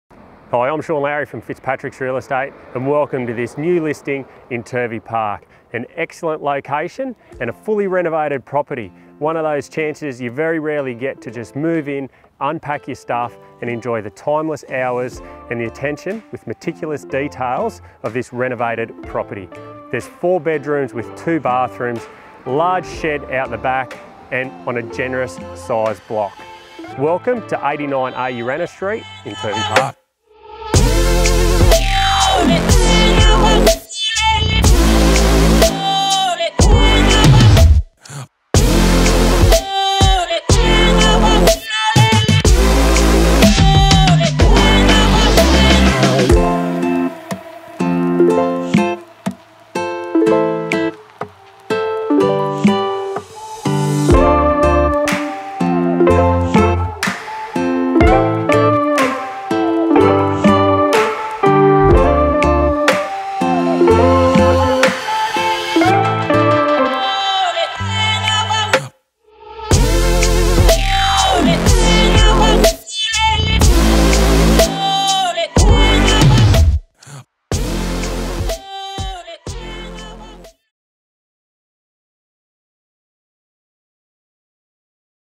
SOLD - 89a Urana Street, Turvey Park, Wagga Wagga
Remodelled to perfection, this gorgeous family home offers a luxurious family lifestyle in a sought after Turvey Park location.